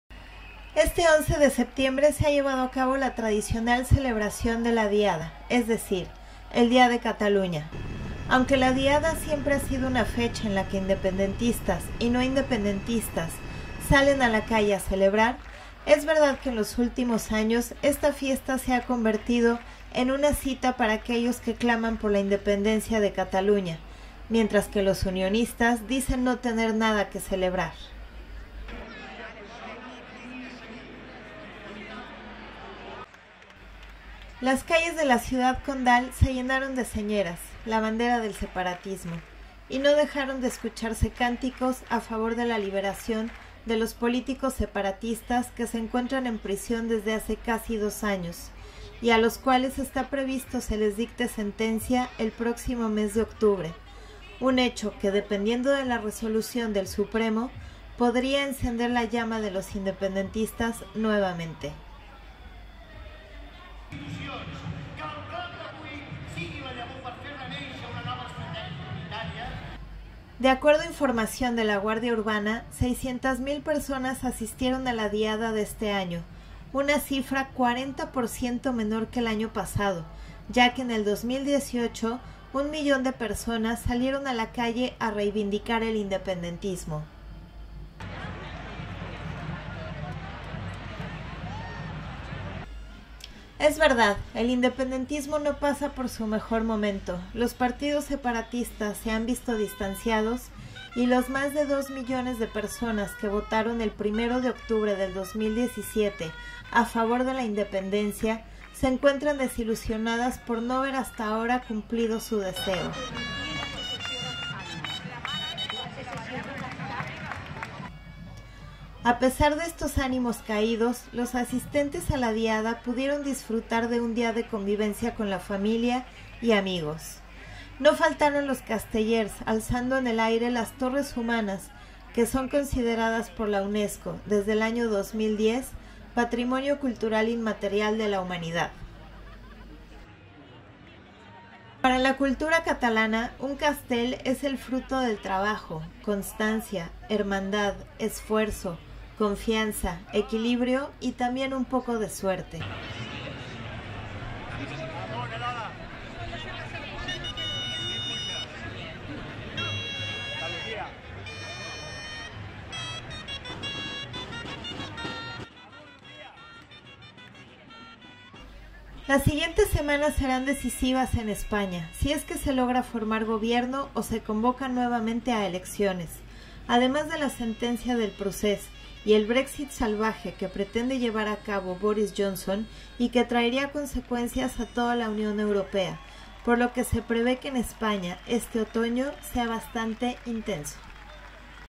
Diada en pausa
600,000 personas celebraron la Diada este 11 de septiembre de 2019, un 40% menos que el año pasado.